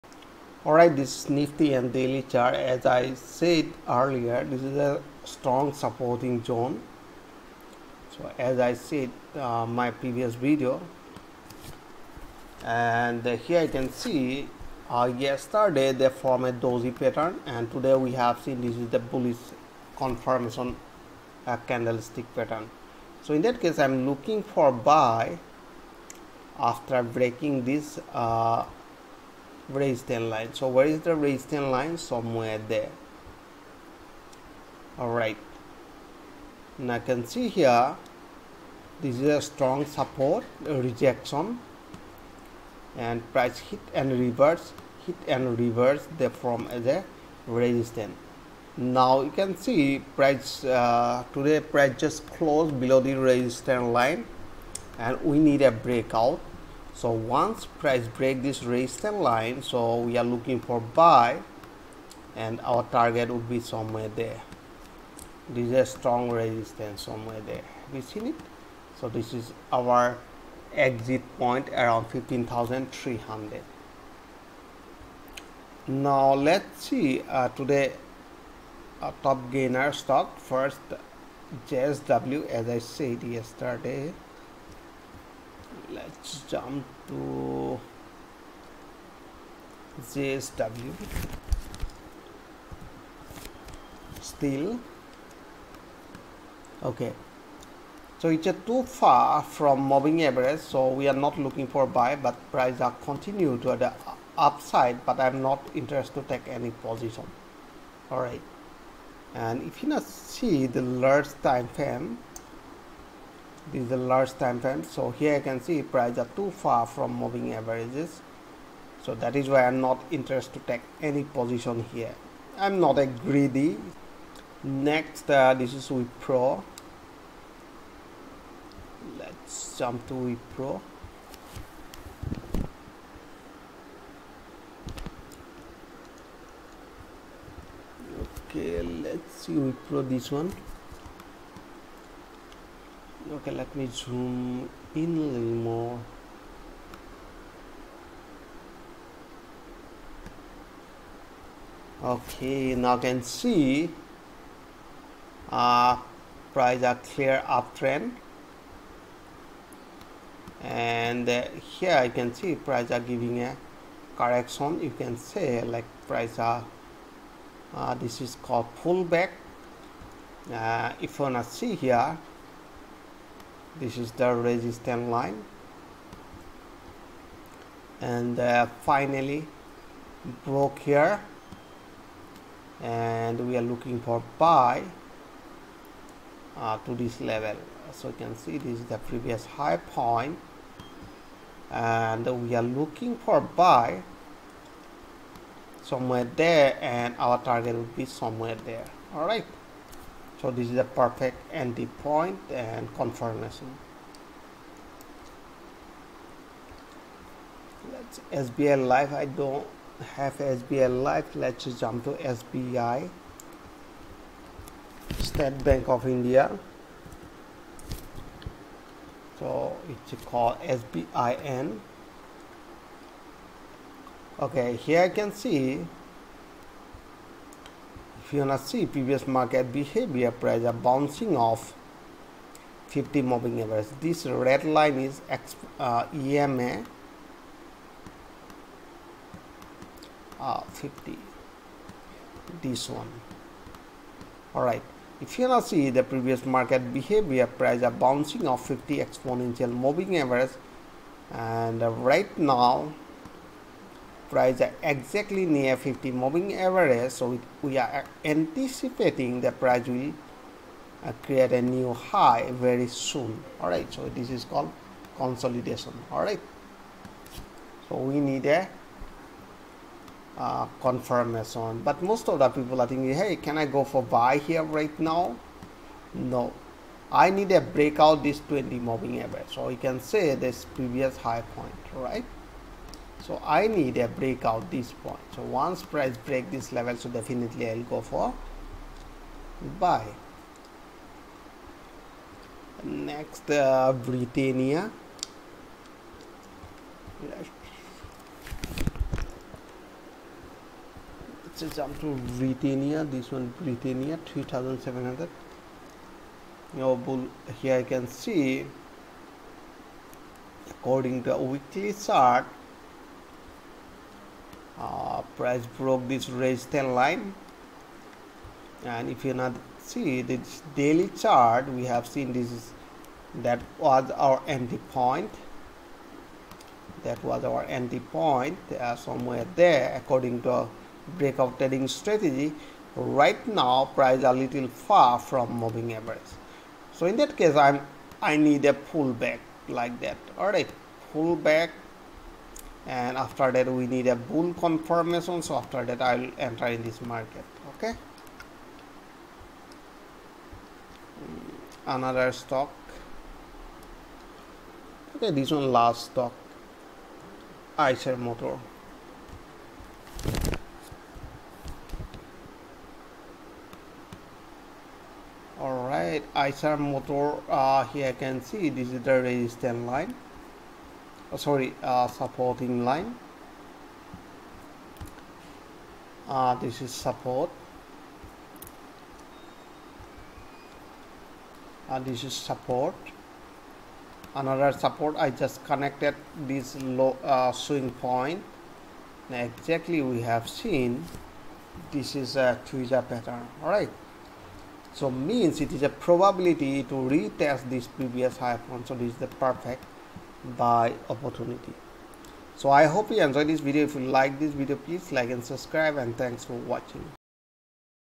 Market Behavior| Top stock analysis | Nifty 50 | Alice blue | 07. 04. 2021
Technical Analysis Of Top Stock | Alice blue | 07 April,  2021

Nifty opened positive on Wednesday and continued its momentum through the first half of the session, but some consolidation was seen in the latter part of the day